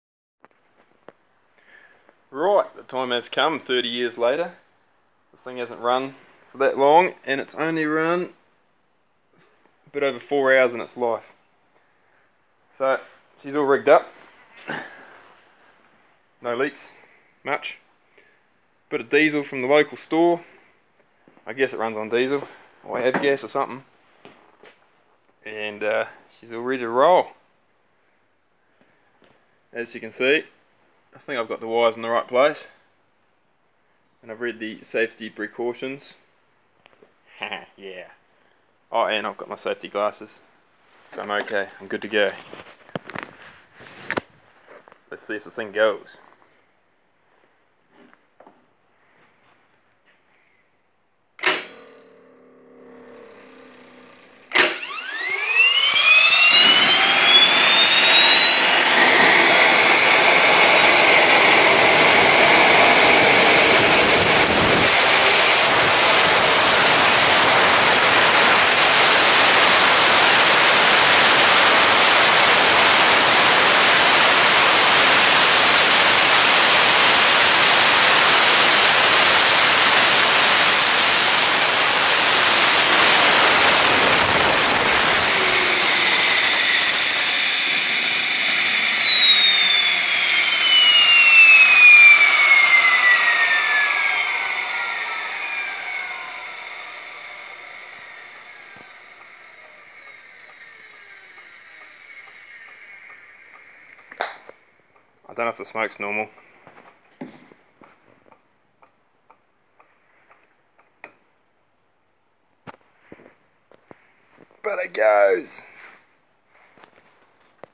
Tiernay TT10 GAS TURBINE ENGINE first start
Had this thing for a bunch of years and finally got a day to put it all together, fired it up and the thing started first go!